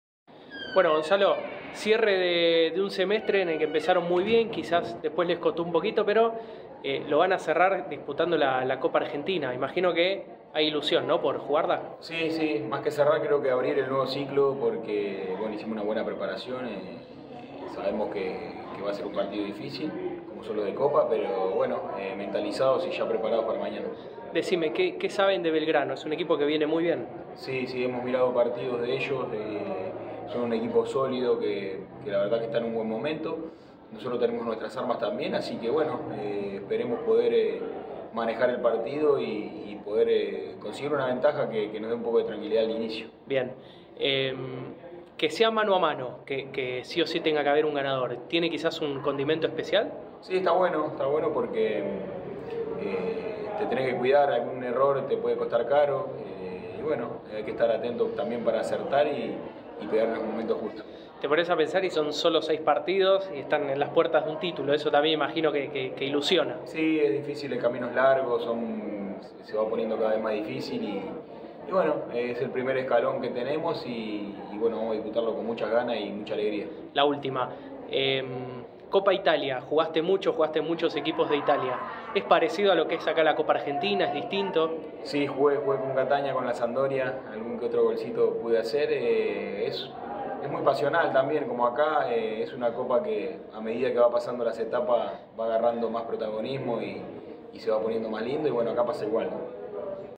Gonzalo Bergessio - Platense
El delantero se refirió al encuentro de este miércoles con Belgrano, por los 32avos.